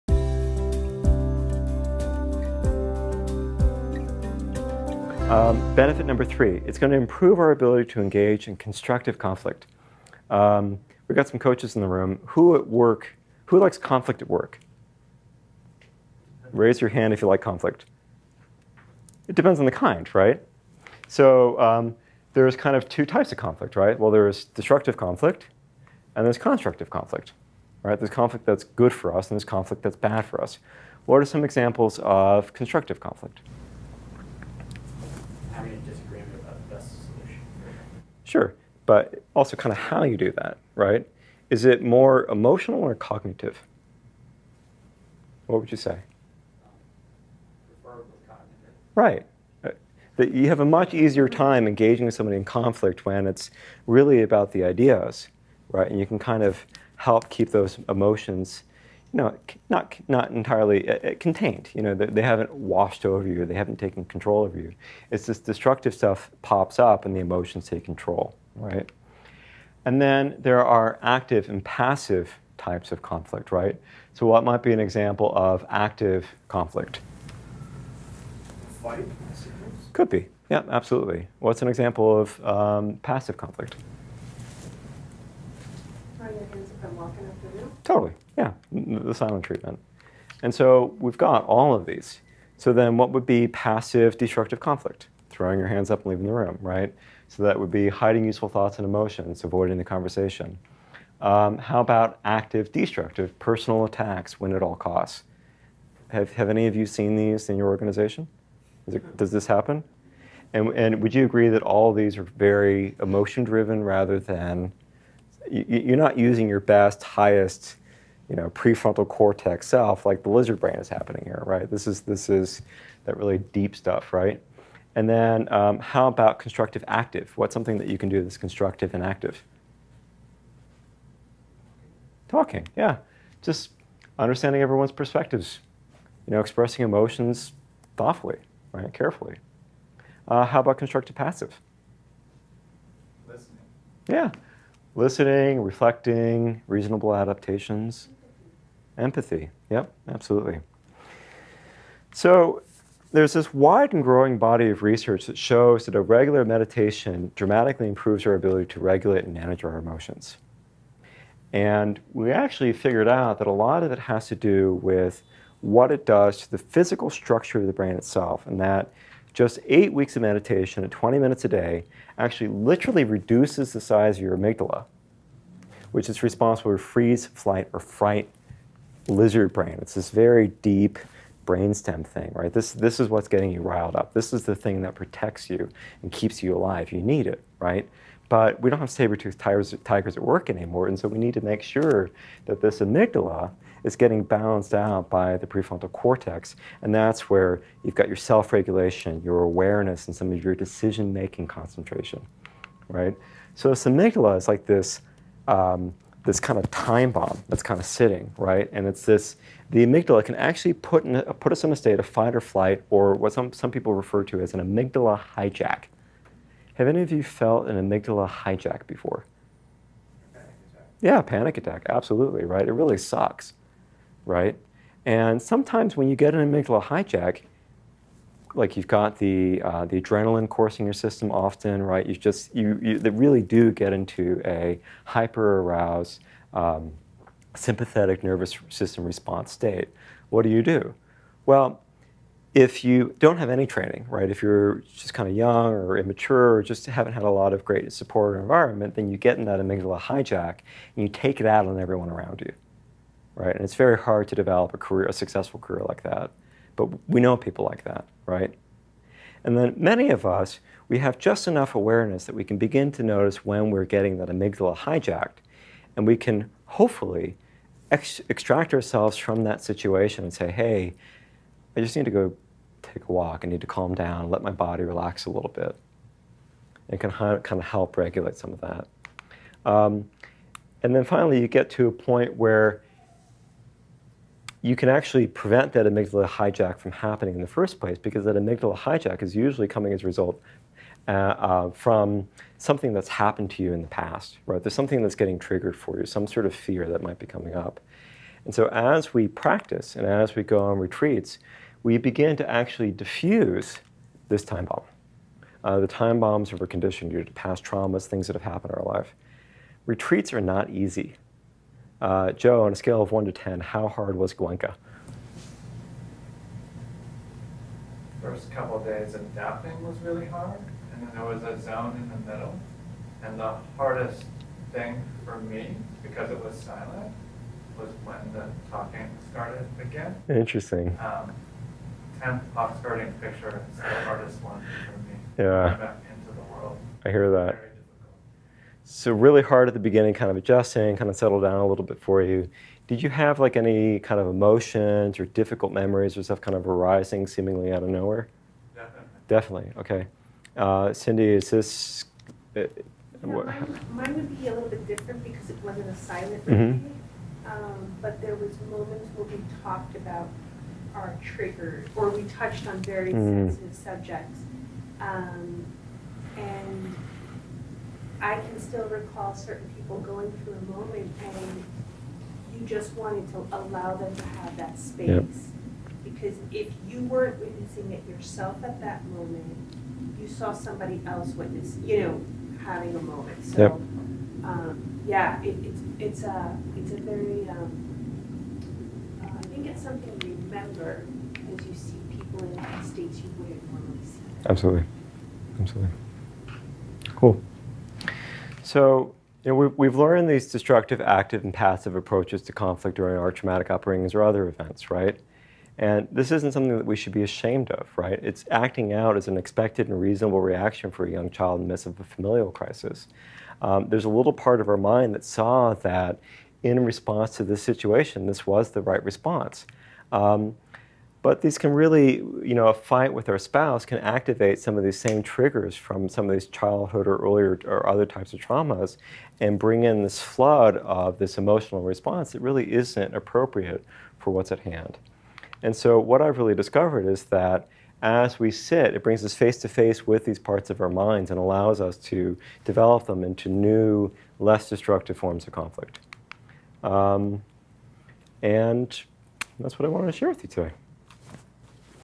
Mindfulness Benefit 3: Better Outcomes During Conflicts
The Conflict Dynamics Model describes conflict as being either constructive (cognitive) or destructive (emotional) and either active or passive. For instance, constructive active conflict might entail seeking to understand everyone’s perspectives, whereas destructive passive conflict might entail avoiding a crucial conversation.

Our ability to remain in cognitive conflict with others relies predominantly on our emotional self-regulation. A growing body of research provides strong evidence that a regular meditation dramatically improves our ability to manage our emotions. In fact, a 2013 study shows that just eight weeks of meditation reduces the size of the amygdala (the brain’s flight-or-flight center) and increases the thickness of the pre-frontal cortex (involved in self-regulation, awareness, and decision-making concentration).

Thus, meditation helps prevent us from winding up in a “flight or fight” or “amygdala hijack” mode when we are in conflict with others by improving our ability to manage our strong emotions when they arise. Better yet, a diligent meditation practice can bring us deep into our psyches and defuse the “ticking time bombs” of our conditioning (due to past traumas, etc).